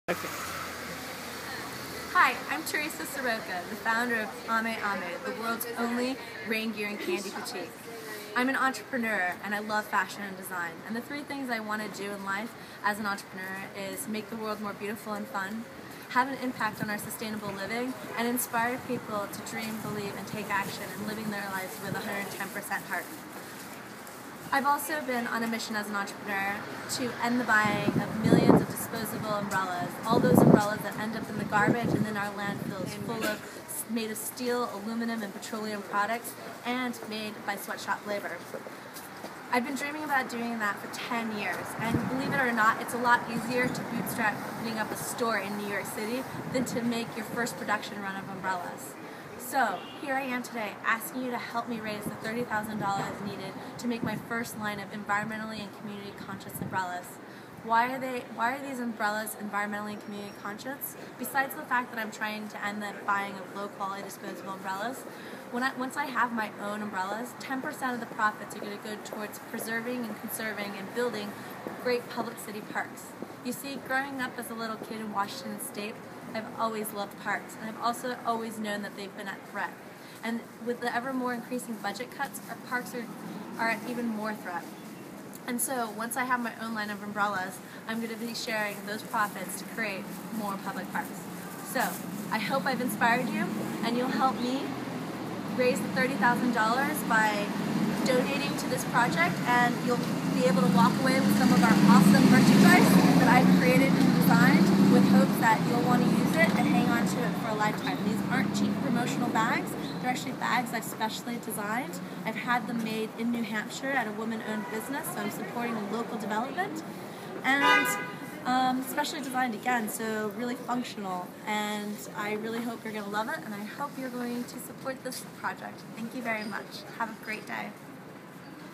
ame-ame-teresa-soroka-indiegogo-video-1
Help support Teresa Soorka, the founder of Amé Amé, produce her first ECC (TM) umbrella, an umbrella that's Environmentally and Community Conscious (TM).

With her ECC umbrellas she intends to share 10% of the profits with building and preserving public city parks around the world.

Teresa grew up in several countries and wishes to make the world more beautiful and fun while changing our living habits to be more sustainable.  And she also hopes to inspire people to dream, believe, and take action while living a life of 110% heart.

is where you can support her campaign.